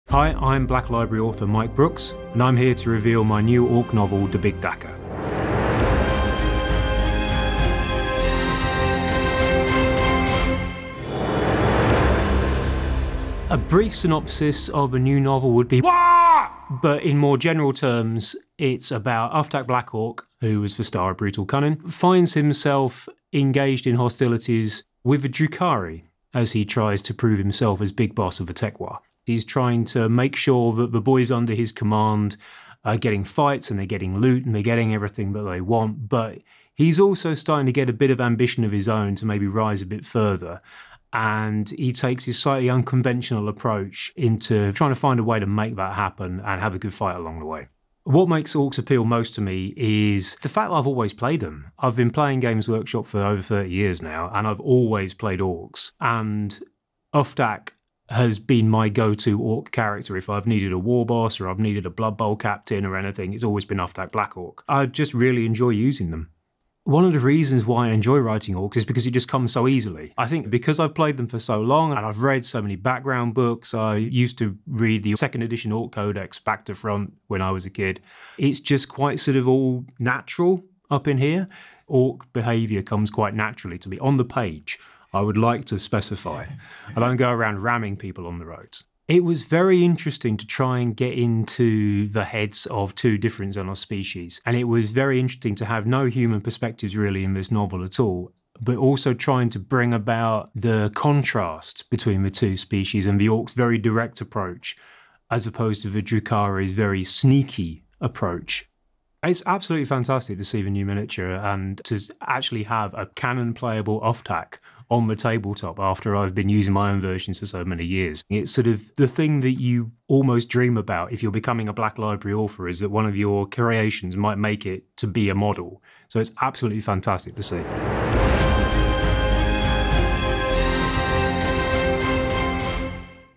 Warhammer Preview Online – Black Library: Mike Brooks Interview
It's time for some krumpin’ in da Dark City as Mike Brooks discusses his new Orks novel.